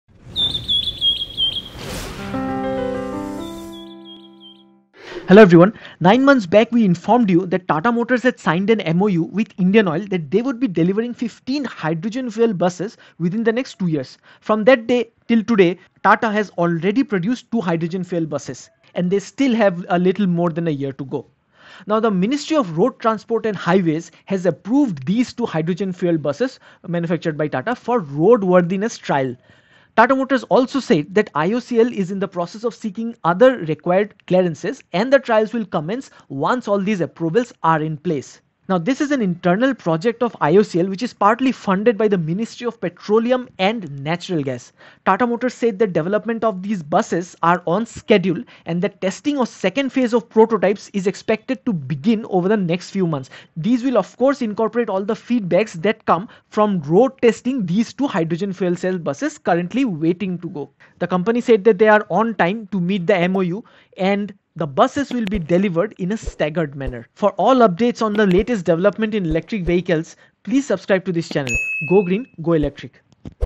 TATA Hydrogen Fuel Cell Bus Ready For Trial Run | Electric Bus | Electric Vehicles | EV India
Tata's hydrogen-based Fuel Cell Buses are approved for Road Worthiness Trials by the Indian Government.  Tata's hydrogen bus gets the approval for roadworthiness trials from govt.

Tata Motors has received approval from the road transport and highways ministry for roadworthiness trials of the company's two hydrogen-powered buses.

The hydrogen-based proton exchange membrane (PEM) fuel cell buses are being manufactured by Tata Motors under a special contract for Indian Oil Corporation Limited (IOCL).

Earlier in June last year, Tata Motors had bagged an order of 15 hydrogen-based fuel cell buses from the IOCL.

Besides, IOCL had also signed an MoU with Tata Motors for two buses for demonstration purposes.

Tata Motors seems to be received approval from the ministry of road transport and Highway (MoRTH) for conducting vehicle trials for these two buses. Tata Motors said that the development of the buses is on schedule.

The company added that the IOCL is in process of seeking other required clearances and the trials will commence once approval processes are complete. IOCL had invited bids for the supply of PEM fuel cell buses in December 2020. This is an internal project of IOCL partly funded by the ministry of petroleum and natural gas.

"Testing of second-phase prototypes is expected to begin over the next few months," the company said, adding that the buses will be delivered in a staggered manner.

Presenter: Sandeep Sen Gupta